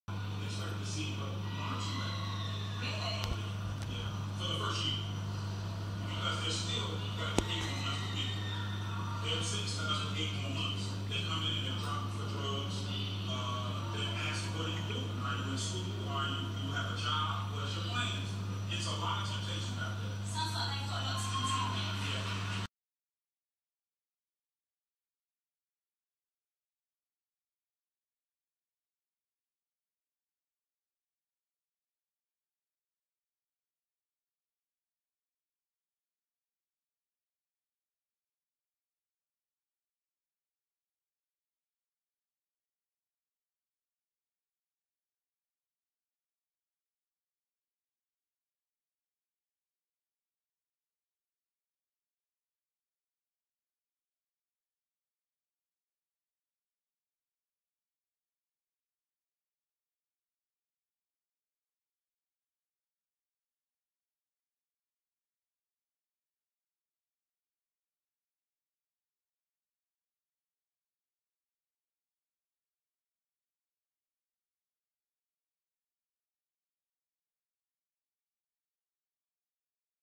Eunicid worm in my tank......
First I didn't know what I was seing.. in the second video.. evil reveals itself...